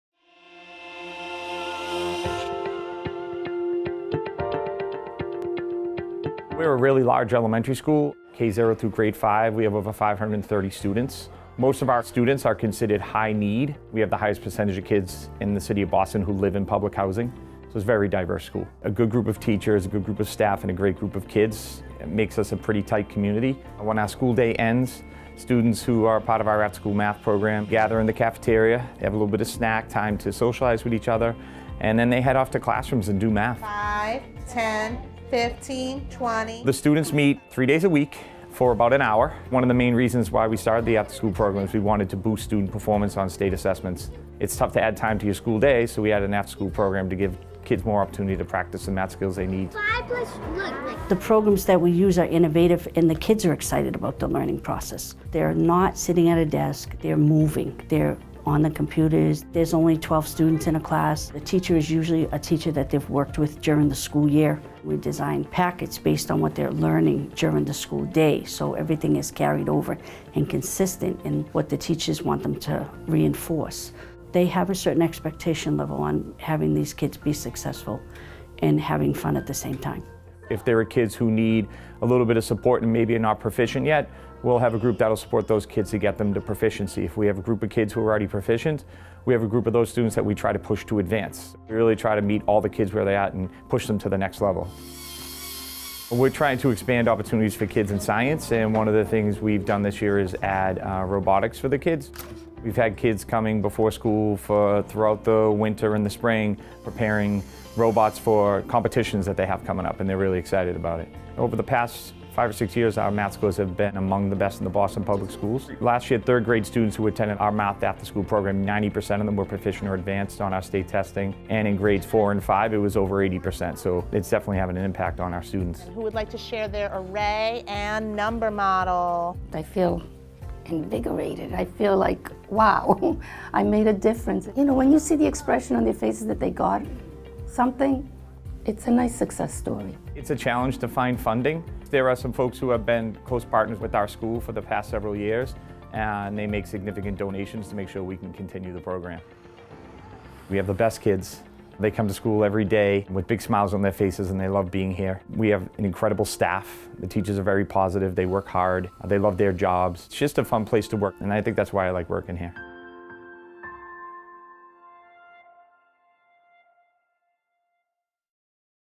Math-Focused Afterschool Tutoring, Harvard-Kent Elementary School
An extra hour of math after school is helping students in Boston’s most impoverished districts booth their math performance.